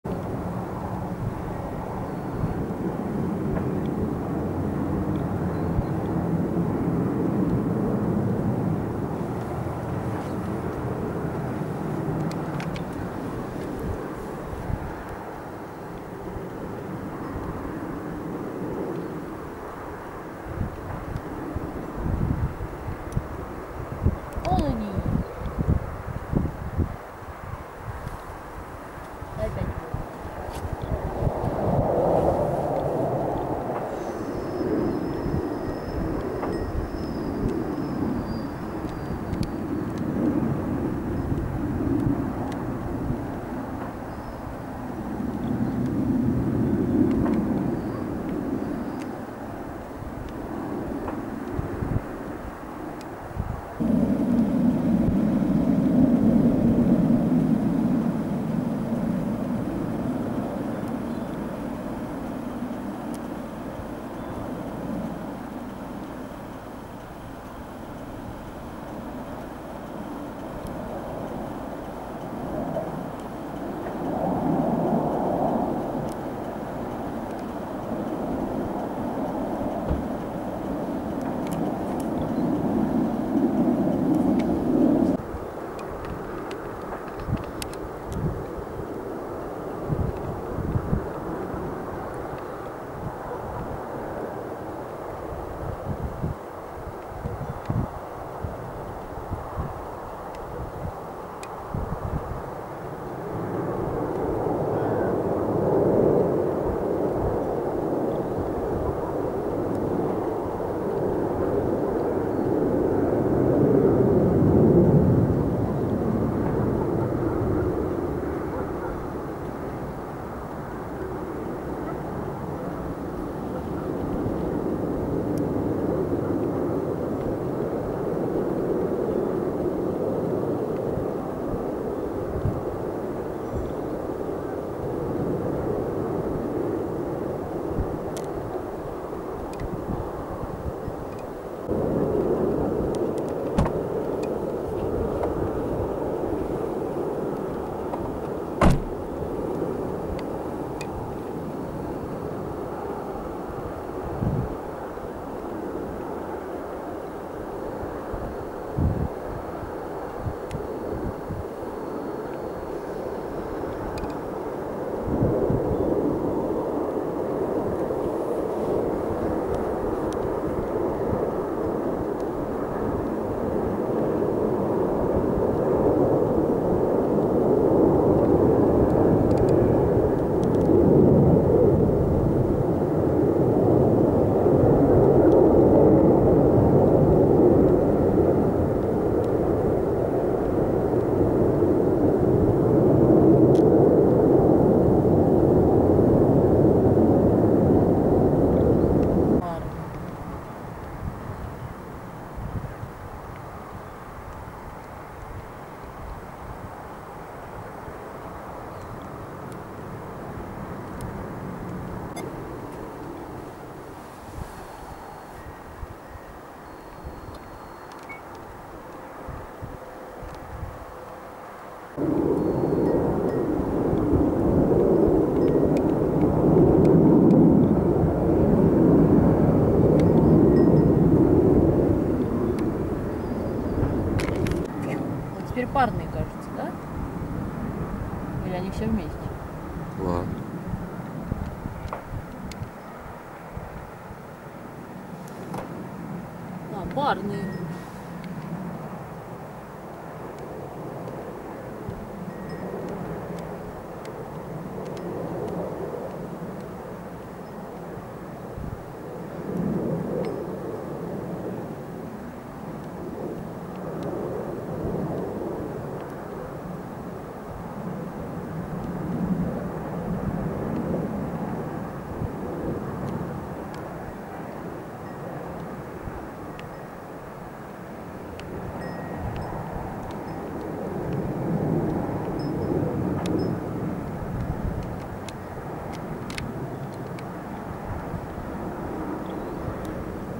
Пилотажная группа «Стрижи» в МУРМАНСКЕ. Смотровая площадка .05.10.19
В честь 103-й годовщины образования Мурманска авиагруппа «Стрижи» показала высший пилотаж над Кольским заливом.